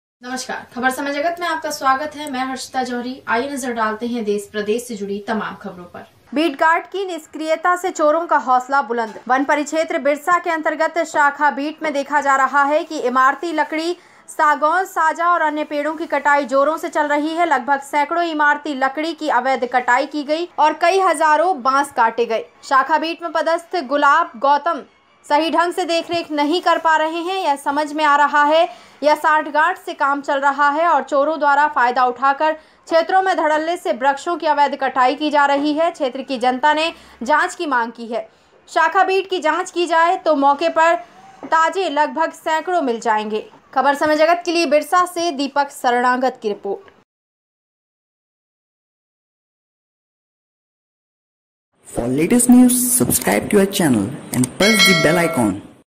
बिरसा - बिट गार्ड की निष्क्रियता से चोरों का हौसला बुलंद
बिरसा से दीपक शरणागत की रिपोर्ट